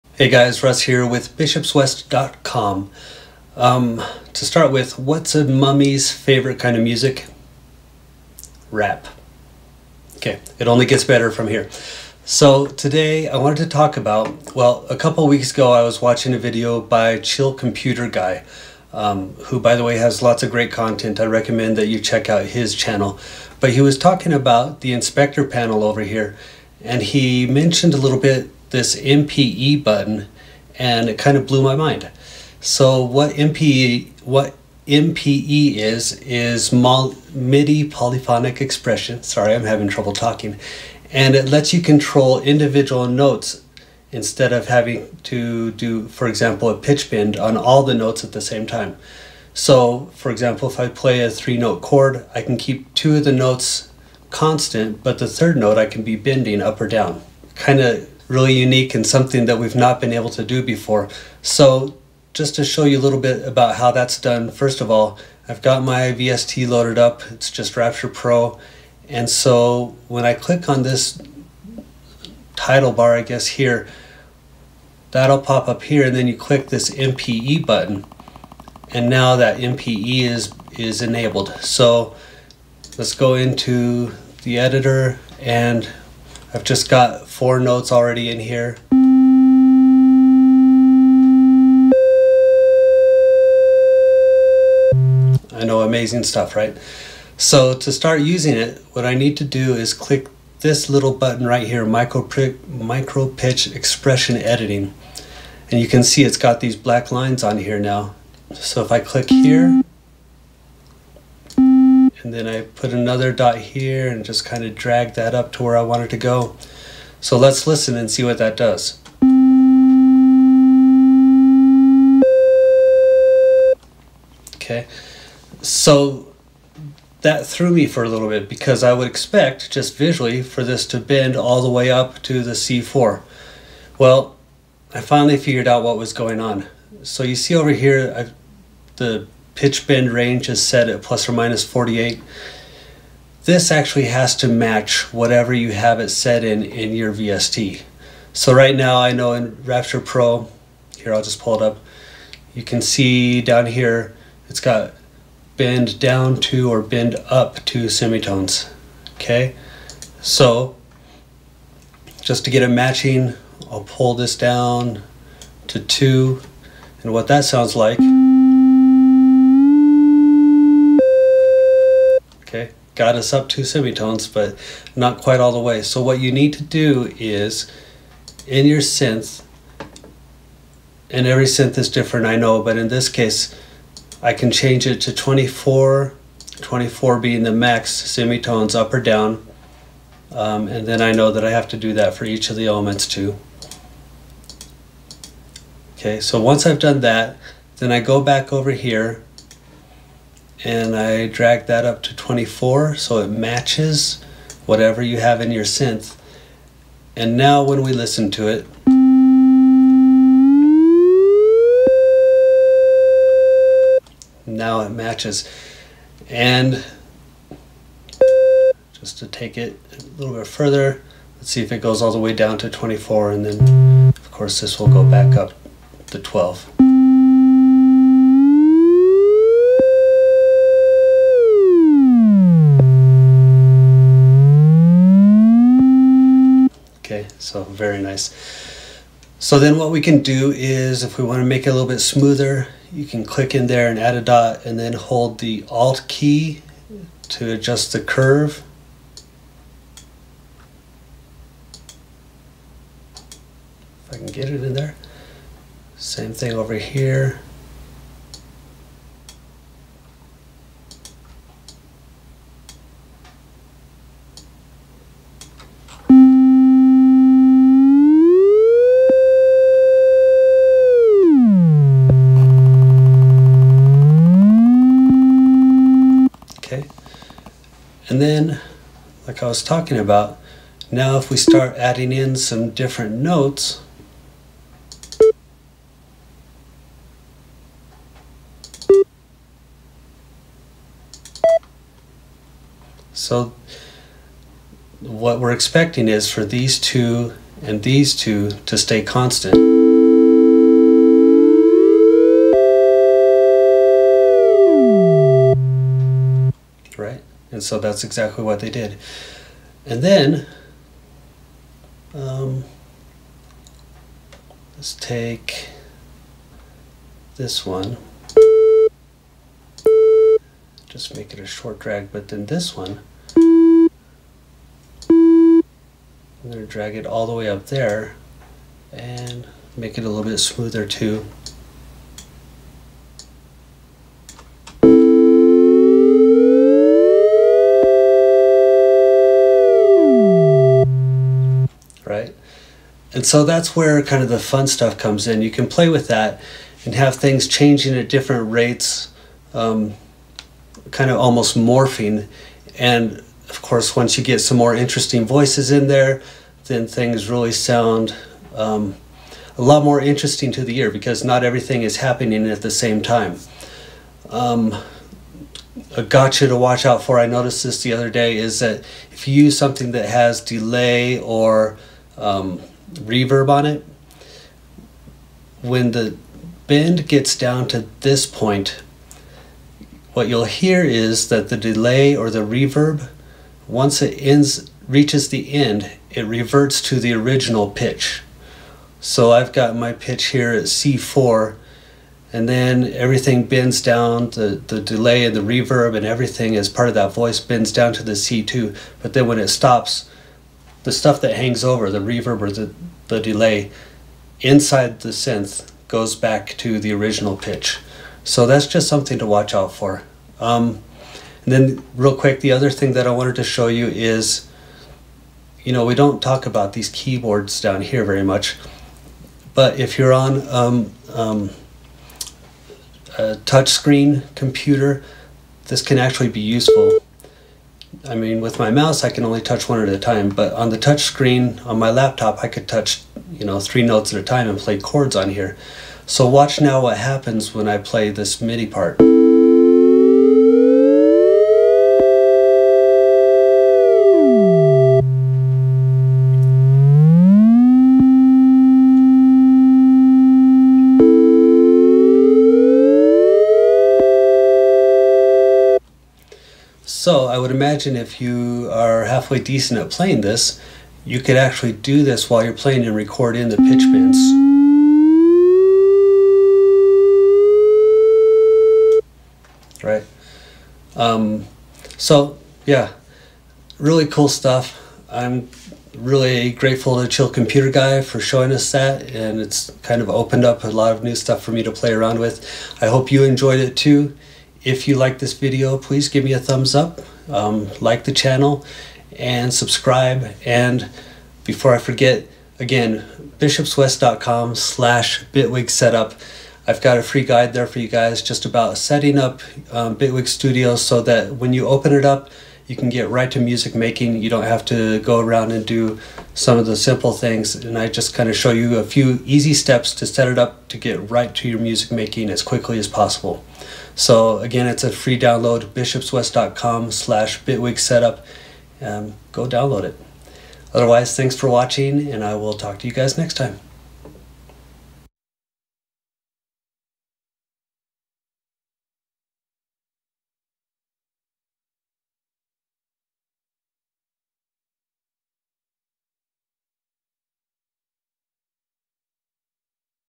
How To: Using MPE with Third Party VSTi's (Bitwig Tutorial)
MPE (MIDI Polyphonic Expression) is a big deal, and it's only going to get bigger. The newest version of Bitwig Studio allows you to use MPE with third party VSTi synths - even those not designed for it - and in this video I show you how to set them up.

Don't forget to download your free guide, "Accelerate Your Music Making in Bitwig Studio." to get the link.